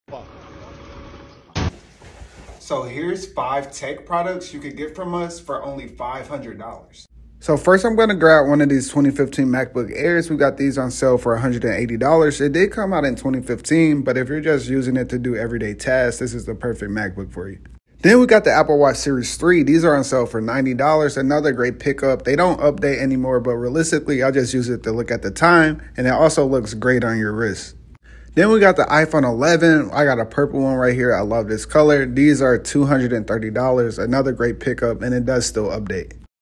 3 Top-Notch Products for Under $500!
Tech lovers, get ready to upgrade your game with these 3 top-notch products for just $500! From a 2015 MacBook Air for only $180 to an Apple Watch Series 3 for just $90, we've got you covered. Don't miss out on these amazing deals! 💻📱